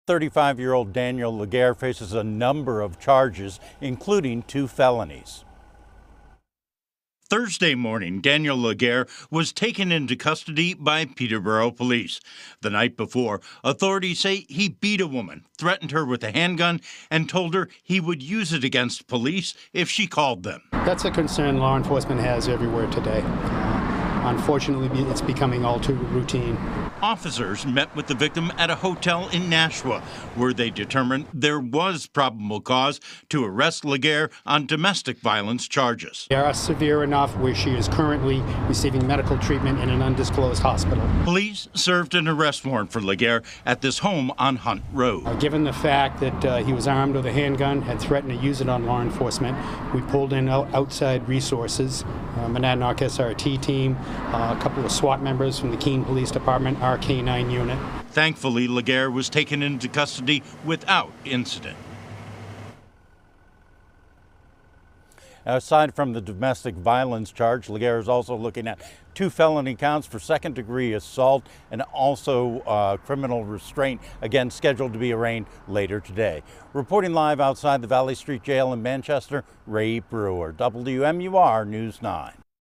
Peterborough man accused of assault due in court Friday
Daniel Laguerre is accused of telling the alleged victim he would shoot at police if she called them.